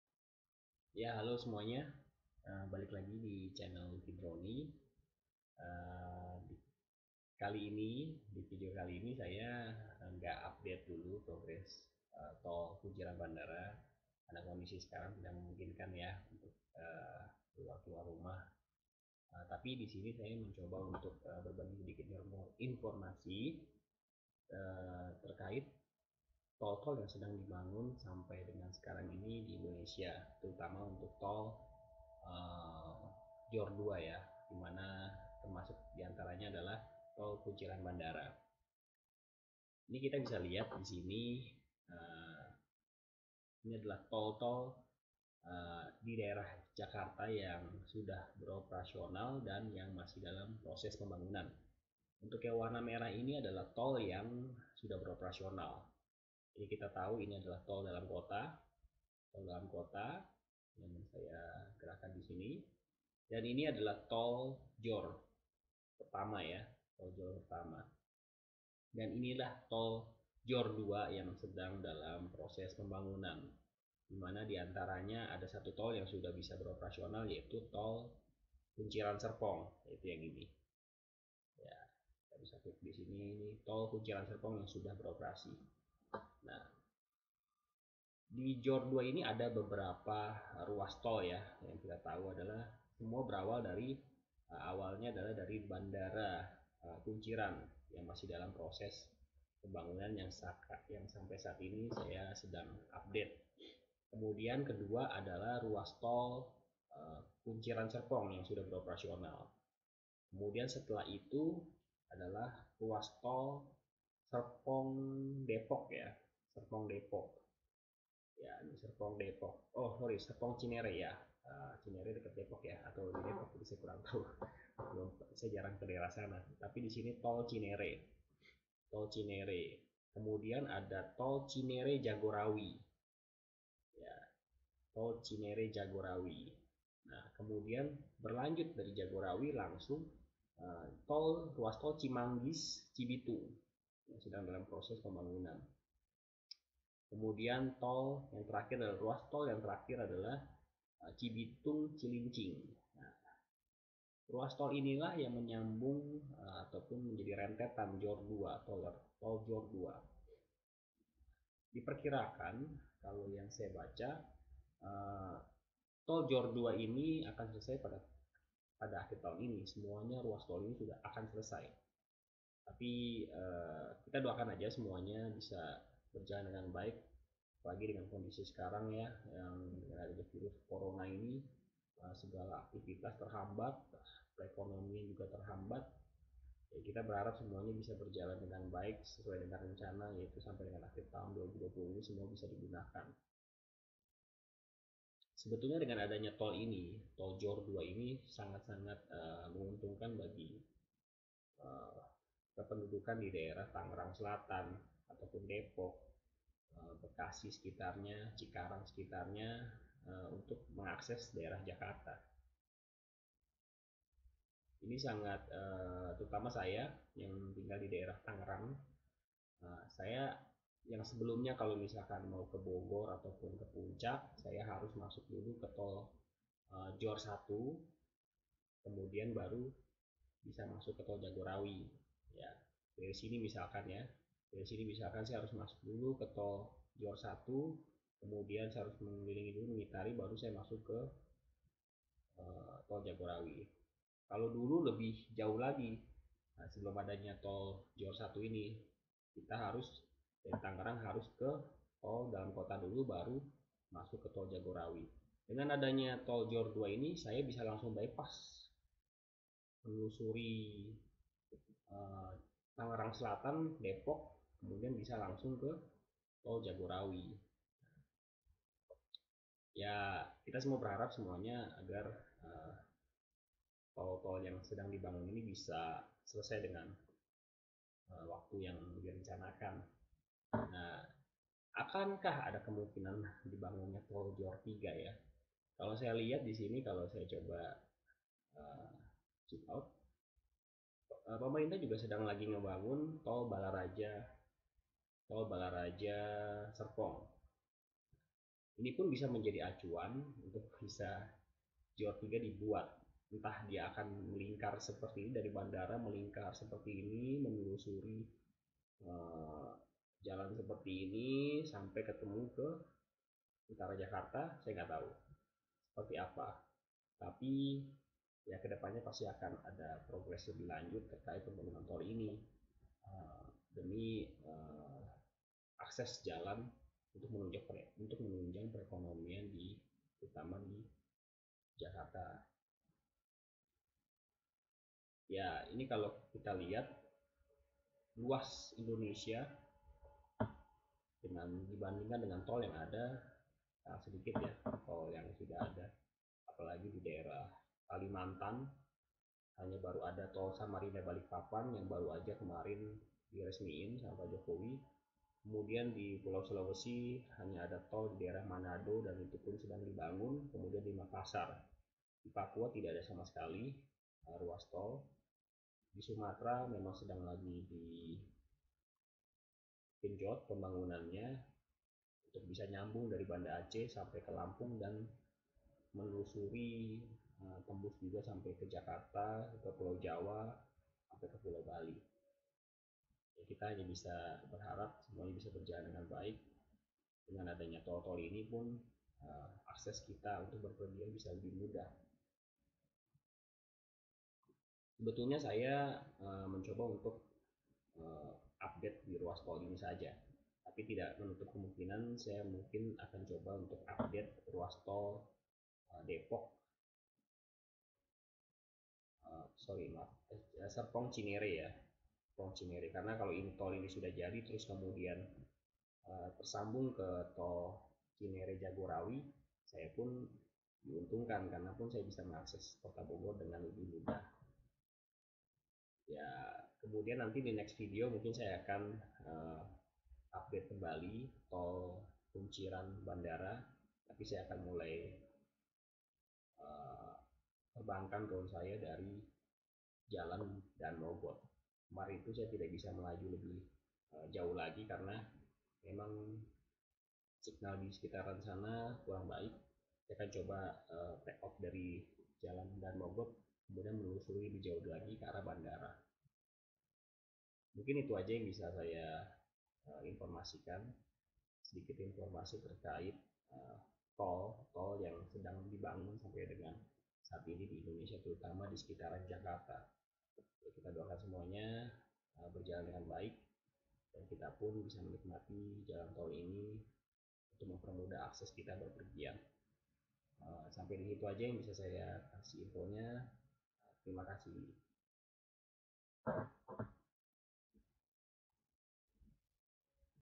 Rute Pembangunan Tol JORR 2, akankah ada JORR 3???
Pada video kali ini, saya mencoba untuk memberikan informasi terkait rute pembangunan dari Tol JORR 2.
- Kunciran - Bandara
- Kunciran - Serpong
- Serpong - Cinere
- Cinere - Jagorawi
- Cimanggis - Cibitung
- Cibitung - Cilincing